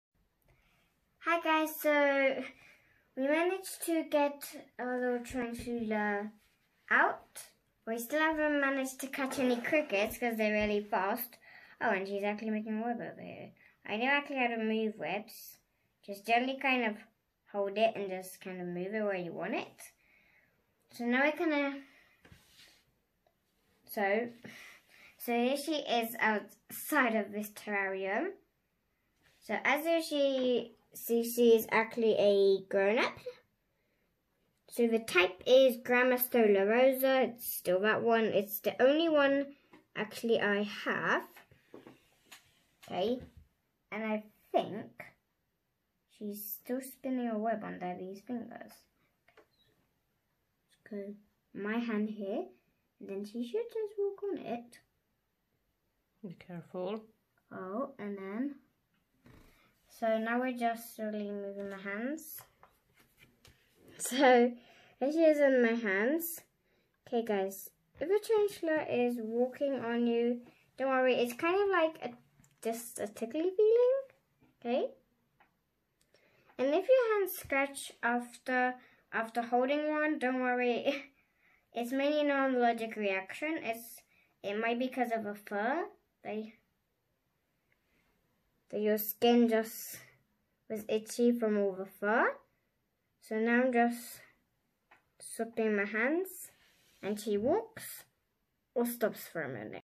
feeding my tarantula Rosy 1/2
feeding a tarantula gramostola rosea